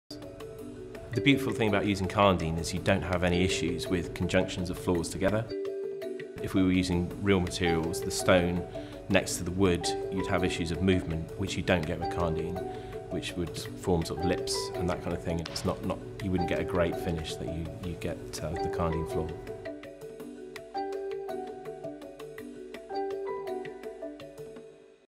Karndean Designflooring | Aqua Restaurant Case Study | Floor Design
Simon Jones from Simple Simon Design in Bristol tells us why he used Karndean's unique geometric Kaleidoscope collection to create Aqua Restaurant's flooring makeover.